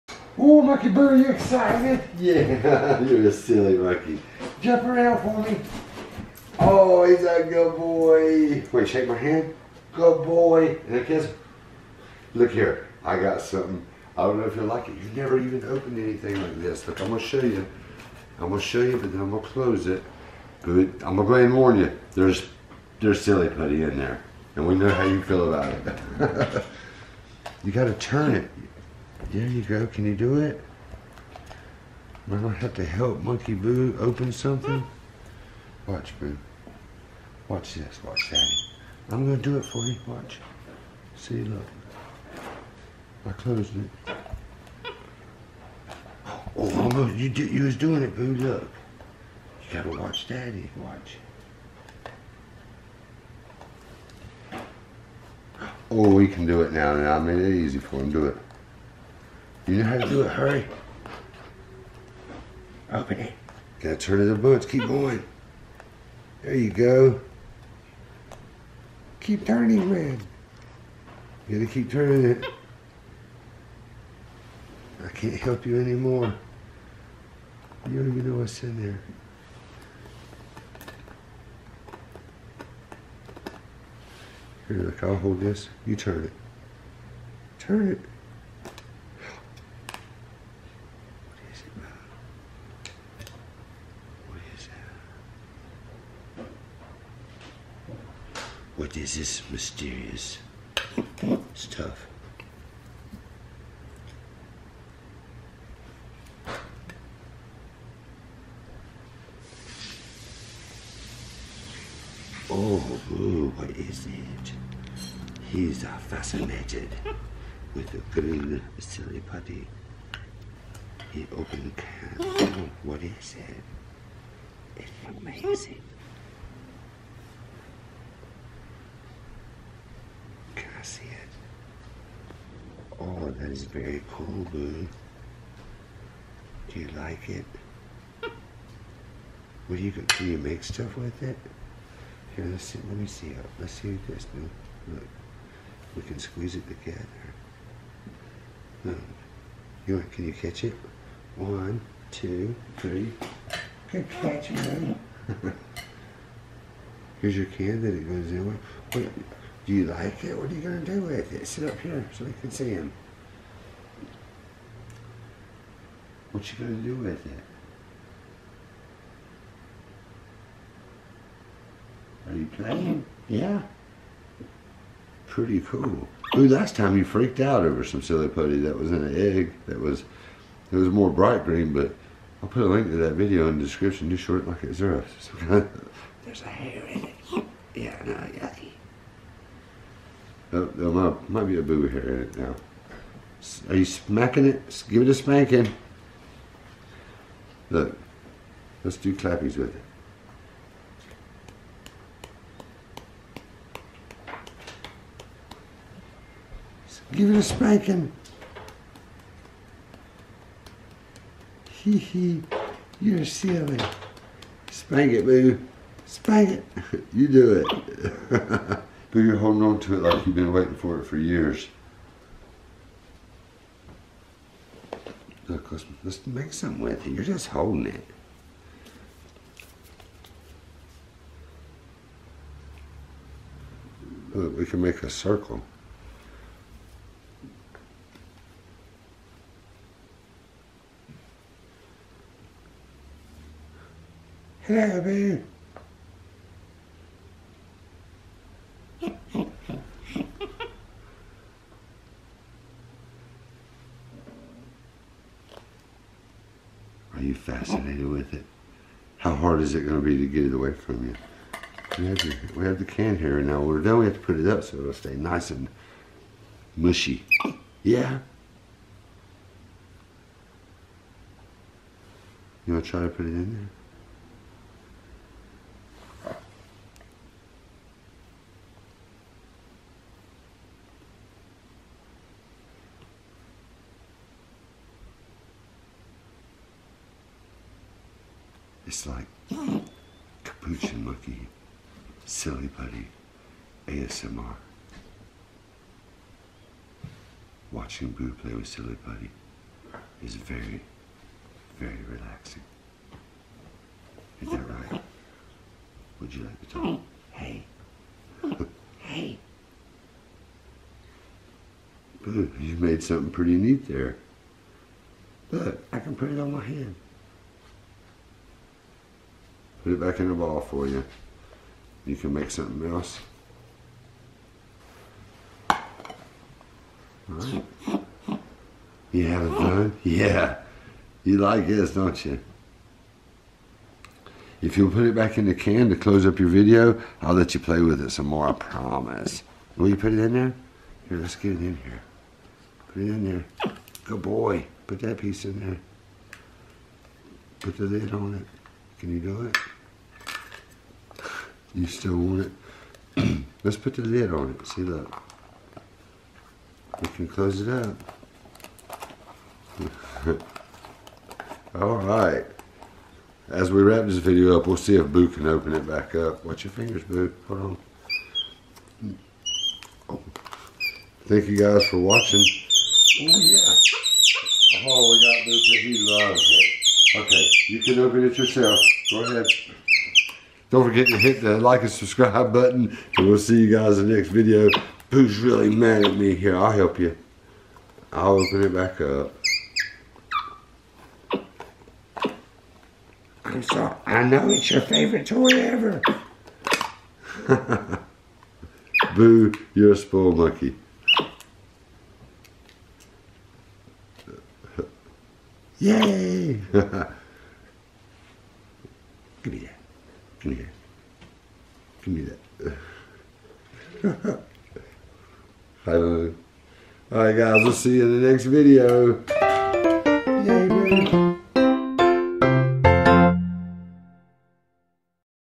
Monkey EXCITED For Silly Putty!
Capuchin MonkeyBoo has decided he LOVES Silly Putty! He even opens a new kind of container he has never seen and pitches a fit when it’s time to put the putty up! CUTE MonkeyBoo!